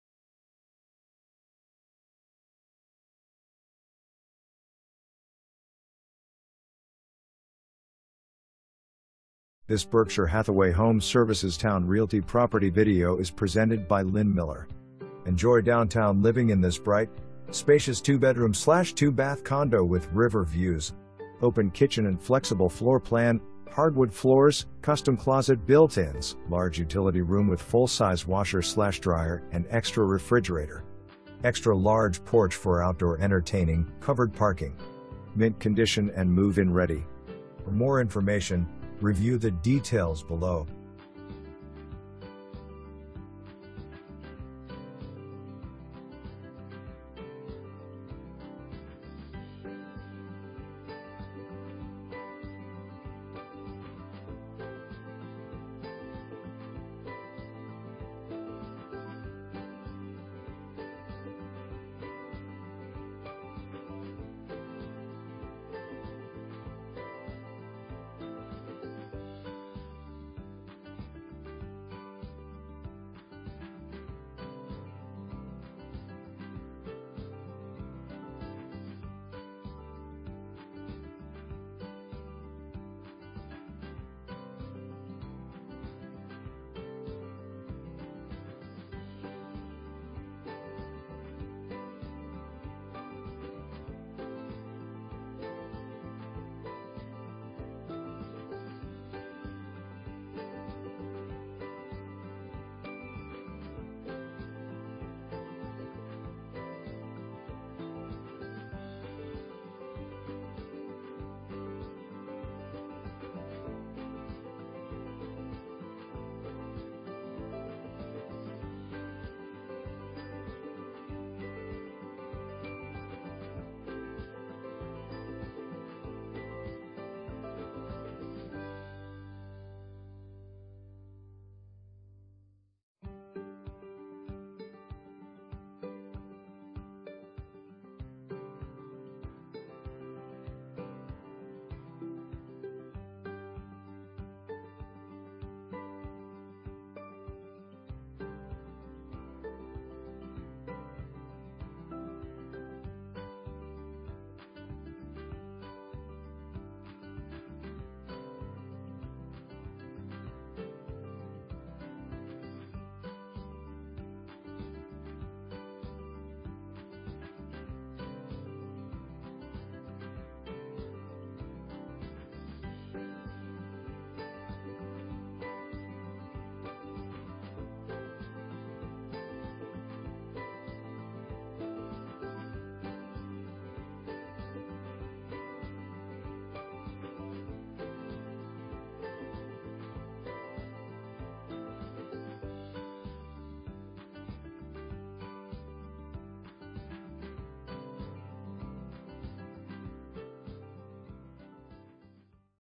Homes for Sale - 239 Duke ST, Norfolk, VA
Enjoy downtown living in this bright, spacious 2BR/2Bath condo with river views. Open kitchen and flexible floor plan, hardwood floors, custom closet built-ins, large utility room with full size washer/dryer and extra refrigerator. Extra large porch for outdoor entertaining, covered parking. Mint condition and move-in ready!

2 full bath

Lin Miller
Berkshire Hathaway HomeServices Towne Realty